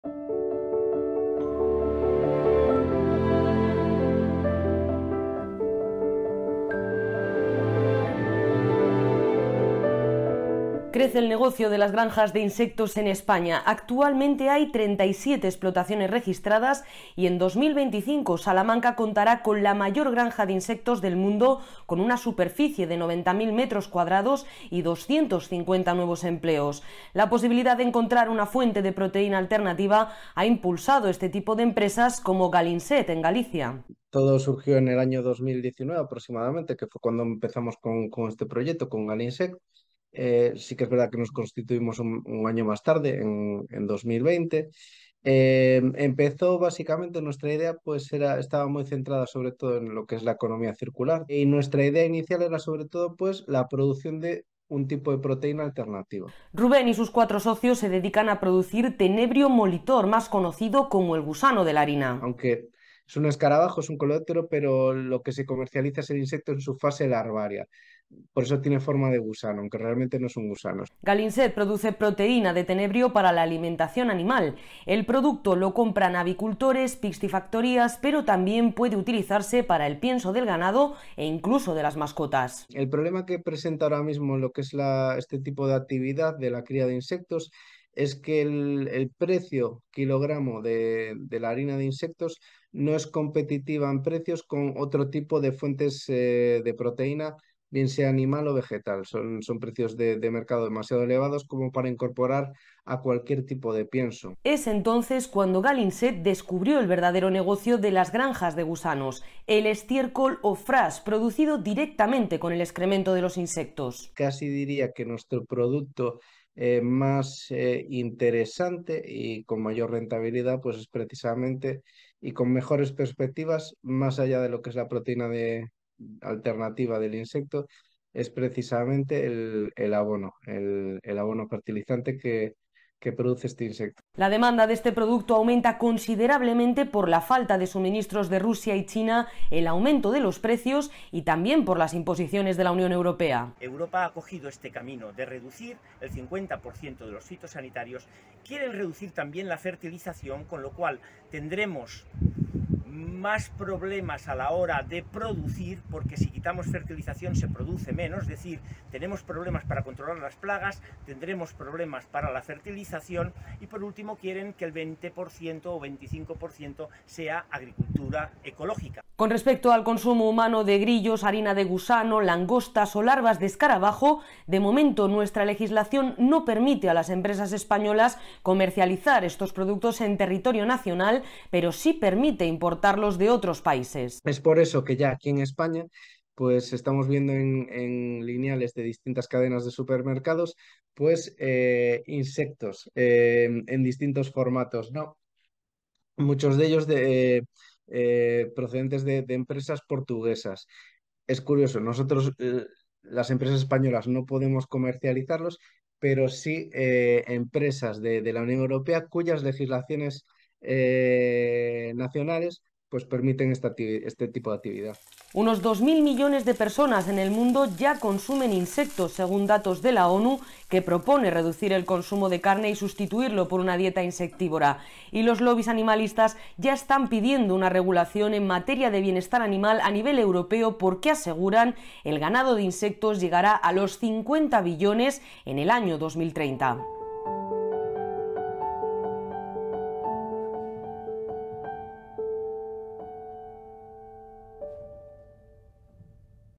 Las granjas de insectos se multiplican en España
España tiene ya 37 explotaciones de insectos registradas, pero el secreto de este negocio no es precisamente que comamos bichos.